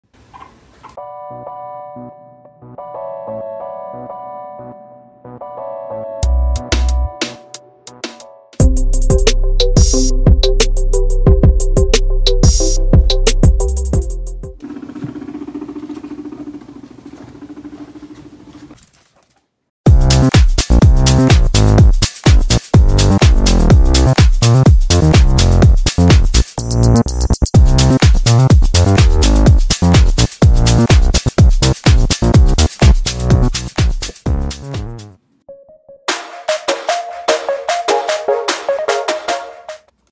🌻GUINEA PIG REACTION: Changing to FLEECE BEDDING🌻
Hey guys! Today we decided to switch to fleece bedding! So far, it’s been a hit⚡️

The fleece provides comfort and ease to their tiny toes 🌸
Apart from the absolute cuteness, fleece liners help prevent infection like bumblefoot. A serious infection that takes place on those tiny toes 🥺
It’s better to be safe than sorry!🍀

Who are Rosie and Daisy?

Rosie - She is an Abyssinian guinea pig who is energetic, adventurous, and curious. She has such a beautiful coat filled with brown, white and black! 🌹

Daisy - Daisy is a mixed breed guinea pig who is “cool,” sneaky and hilarious! Her coat is unique including white, black and a hint of peach!🌼

Wow! You read up to here? Thanks!😊